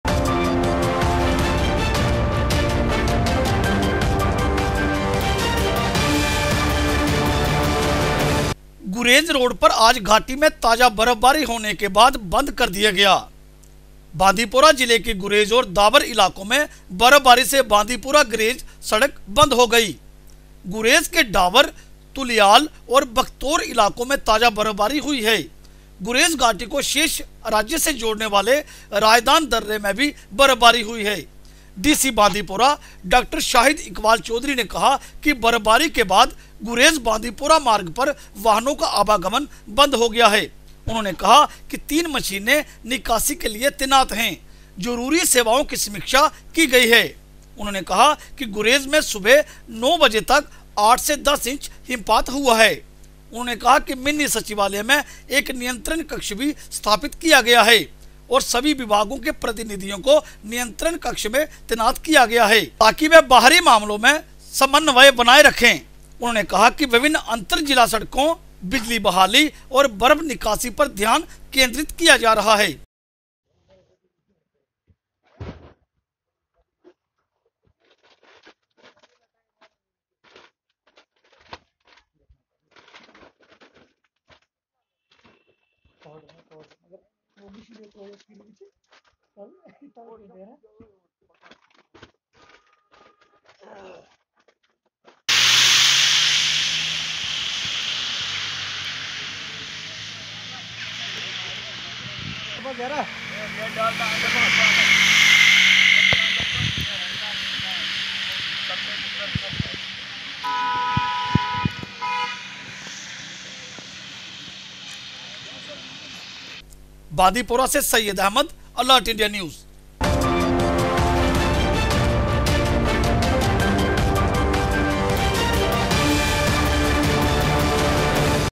Bandipora-Gurez road closed following fresh snowfall
Bandipora, Jan 02: Bandipora-Gurez Road here on today was closed after Valley received fresh snowfall. Snowfall in Gurez and Dawar areas of Bandipora district has led to closure of Bandipora-Gurez road.

Reports reaching that Dawar, Tulial and Baktore areas of Gurez received fresh snowfall. Reports said that Razdan Pass connecting Gurez Valley with rest of the state has also received snowfall.

DC Bandipora Dr Shahid Iqbal Choudhary said that, the traffic on Gurez-Bandipora road closed for the vehicular traffic following snowfall.

He said that 3 machines deployed for clearance, and men and machinery are on the job. Essential services reviewed for District. He said that Gurez valley received 8-10 inches feet of snow till 9AM.

Dr Shahid said that, one snow cutter is working at Kanzalwan Bagtore road, Two at Dawar while one machine has been deployed in Tulail area.

He said all internal routes of Gurez zone have been cleared. Power water supply and other essential commodities are still working.

Meanwhile the Deputy Commissioner Bandipora reviewed the preparedness of any eventuality during snowfall. He directed the officials to keep track of the public greviances and resolve them at earliest and ensure regular supply of essential commodities.

He said the 33KV lines pertain to STD Gandarbal/Bandipora are running smoothly. Dr Choudhary said the administration is ready to tackle with any kind of situation and that the men and machinery has been put on standby mode. He said the snow clearance machines have been put on alert  at various spots in the district so that snow is cleared from the roads at earliest.

He said a control room has also been established in Mini-secretariat and representatives from all the department have been deployed in the control room to maintain close coordination in case of exigencies. He said focus is being laid on snow clearance on various inter-district roads and power restoration.
News by : Syed Ahmad Jk